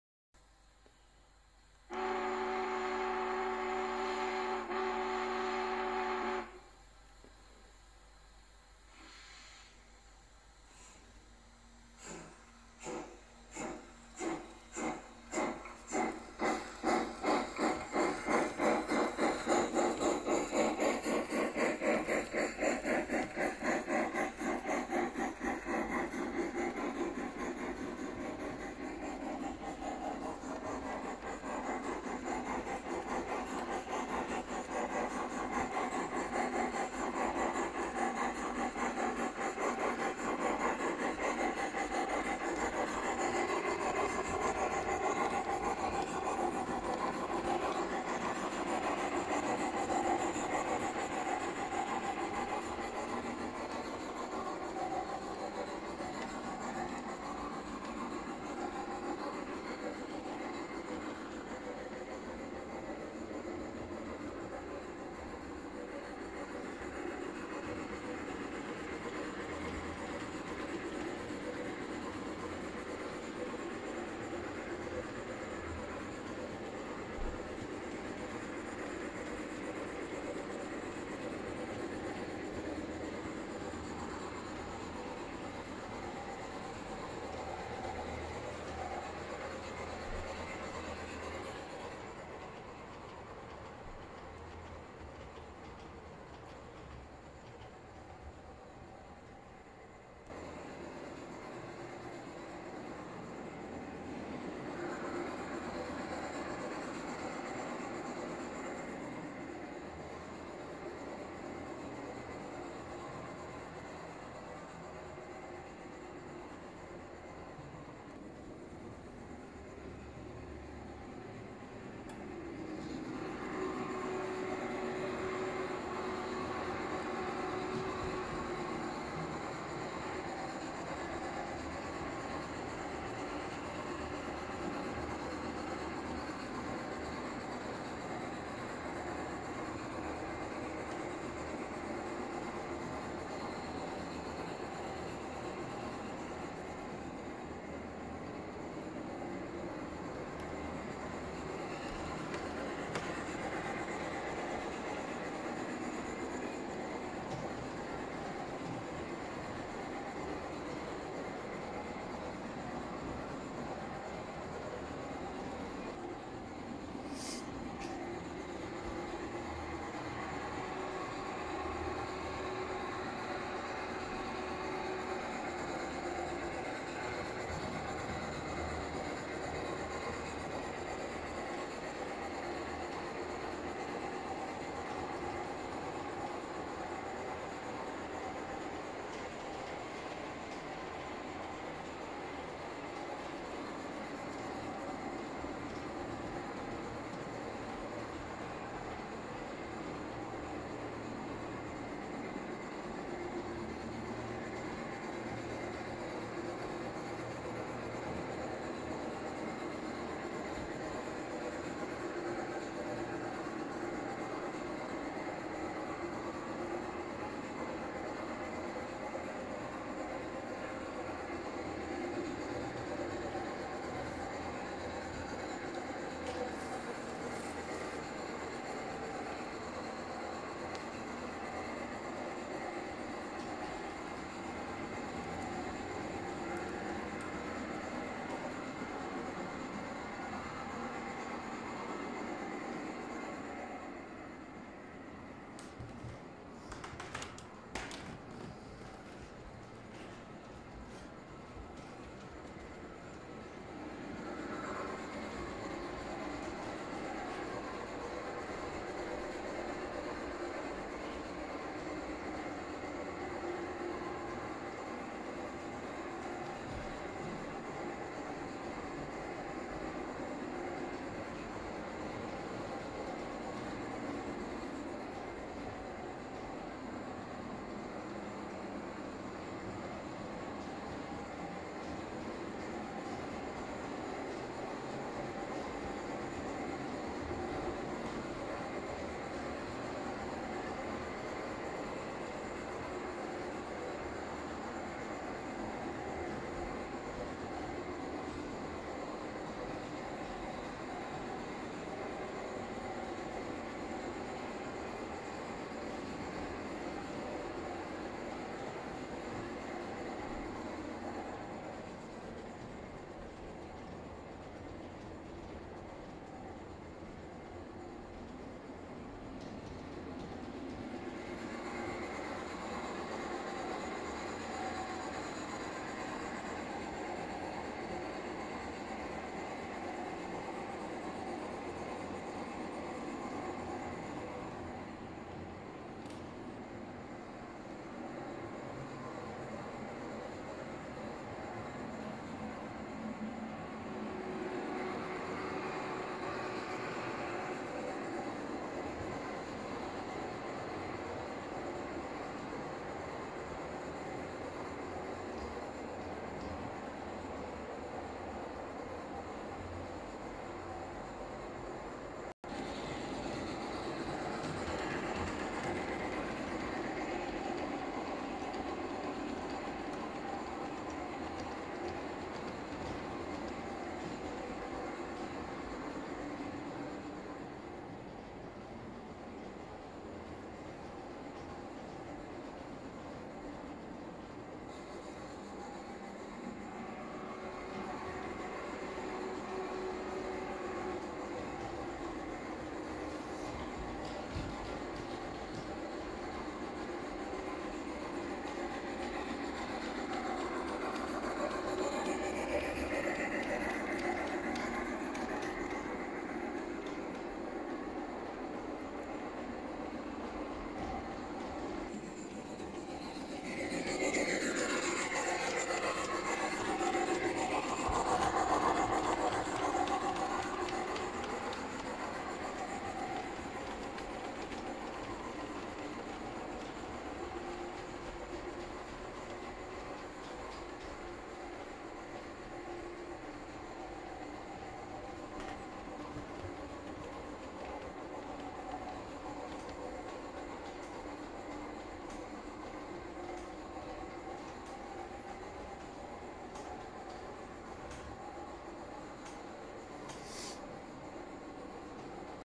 lgb weihnachtszug
Hier wieder ein kleines update und mein Weihnachtszug
Allen da draussen schöne Festtage.
Gruss Patrick